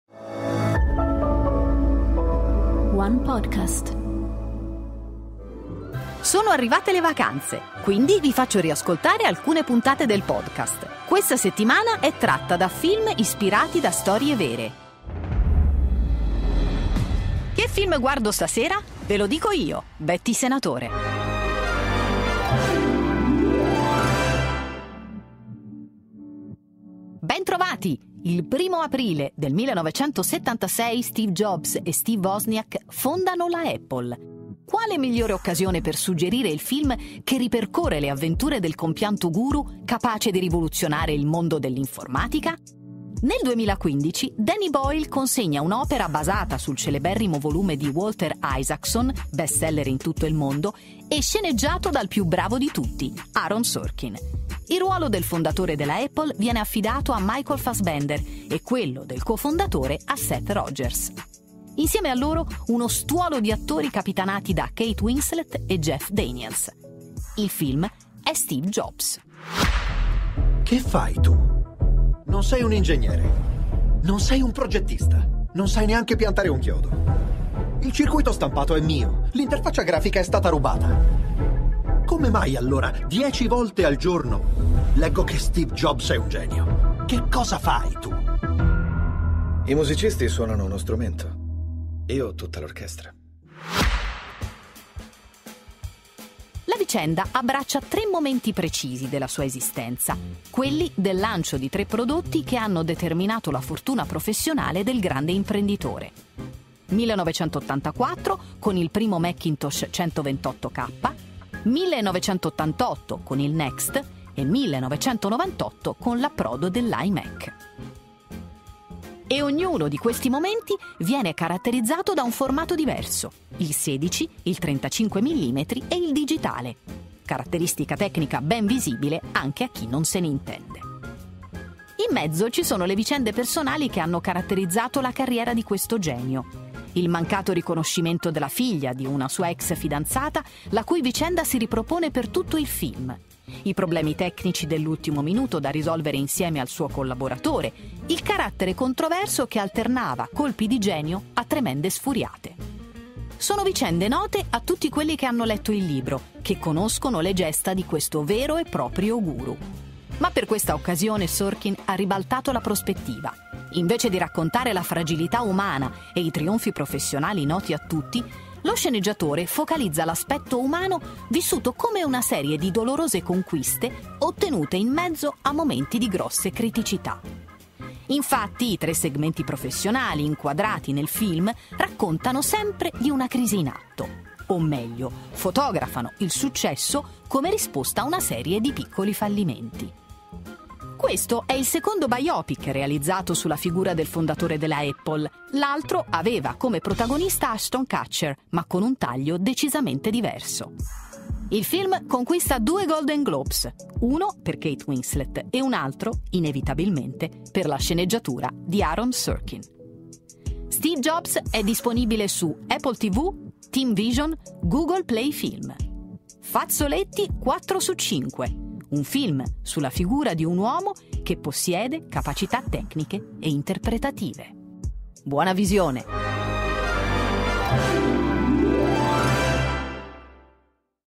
Best of #78 - Steve Jobs
I film di questa settimana sono tratti da storie vere. Per i mesi di luglio e agosto, Betty Senatore ripropone alcuni dei migliori titoli di “Che film guardo stasera?”.